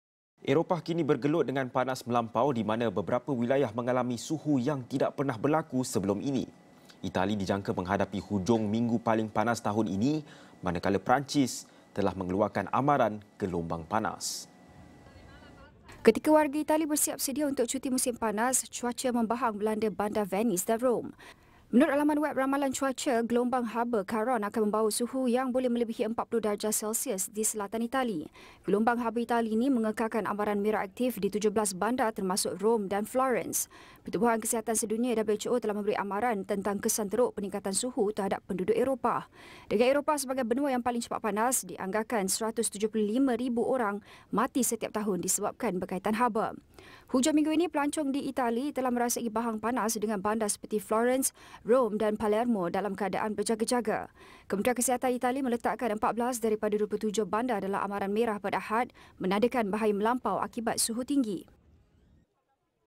Itali dijangka hadapi hujung minggu paling panas tahun ini
Kementerian Kesihatan Itali meletakkan 14 daripada 27 bandar dalam amaran merah pada Ahad, menandakan bahaya melampau akibat suhu tinggi.